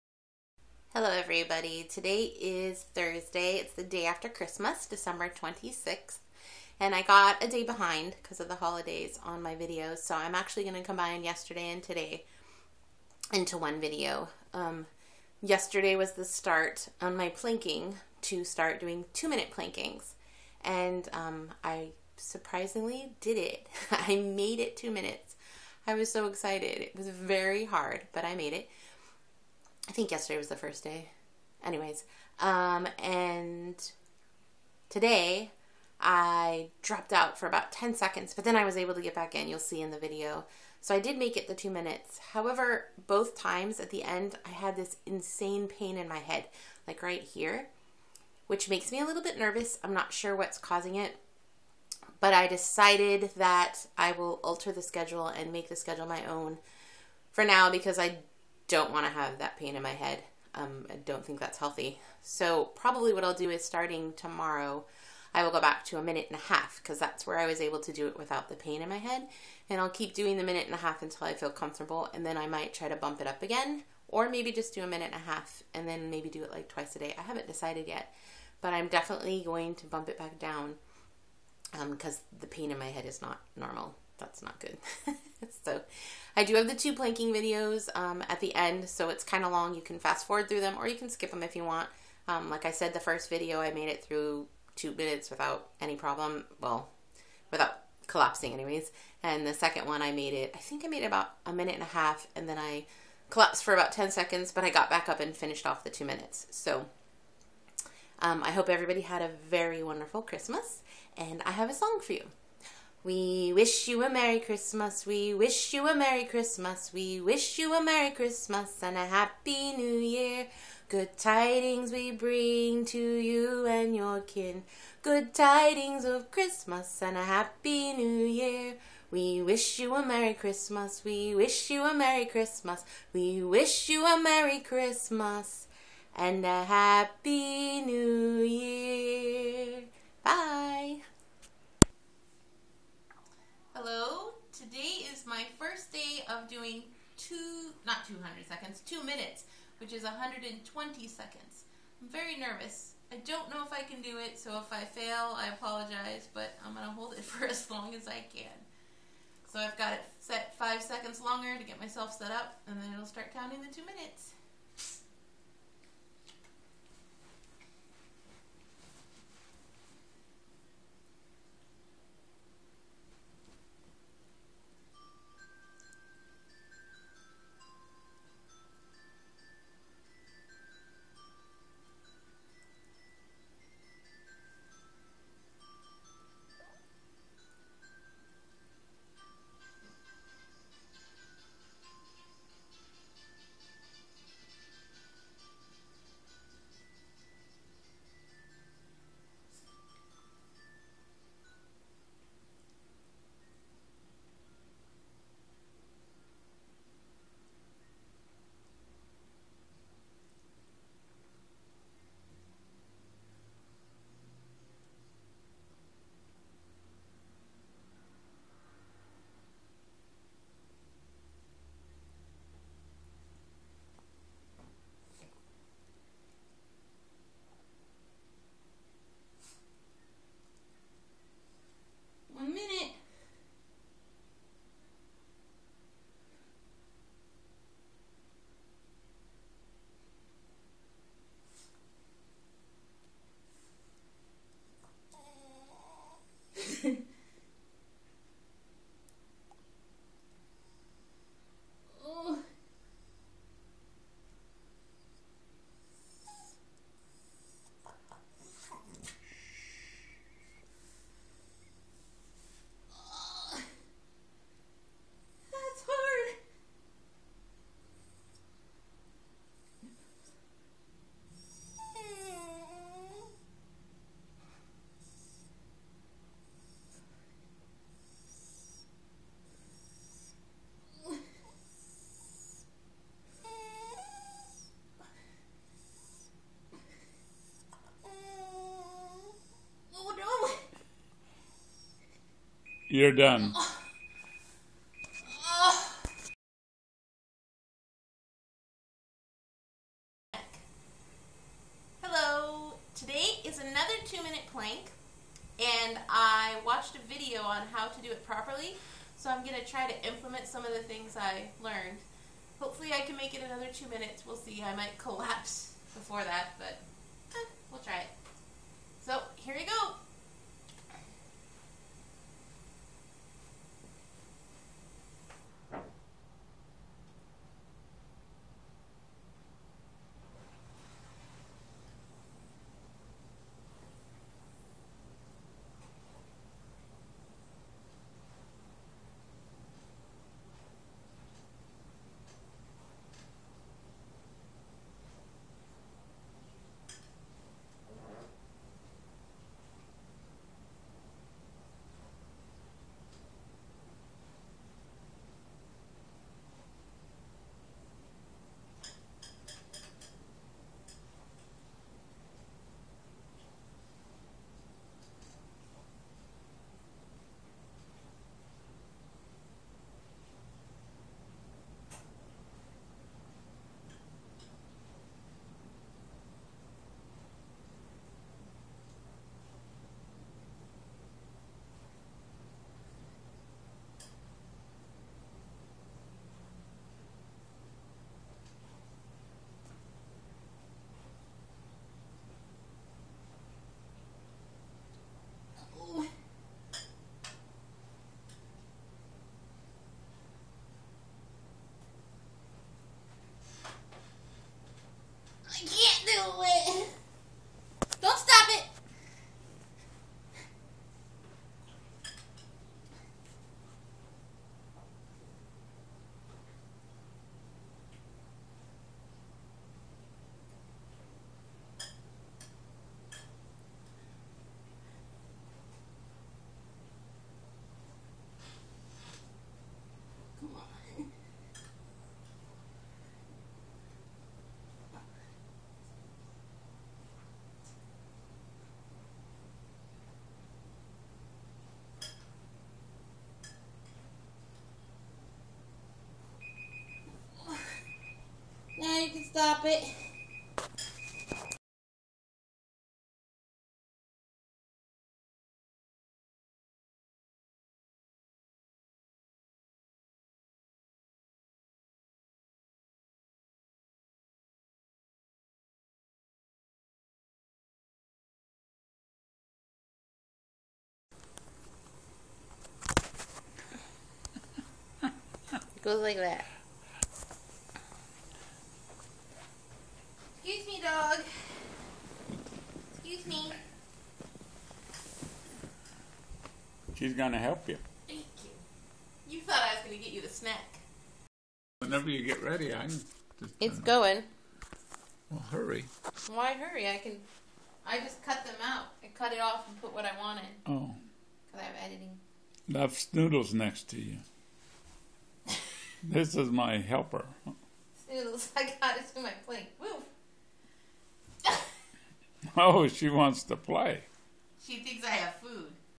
Days 179 & 180 - Plank days 16 &17
Christmas food -
smoothie - banana and peanut butter
and lots of other junk!

December 24 food -
Veggie Chili and crackers
handful of almonds and popcorn chips
2 small flatbreads with vegan pepperoni and lettuce
Homemade bread with butter
dark chocolate chips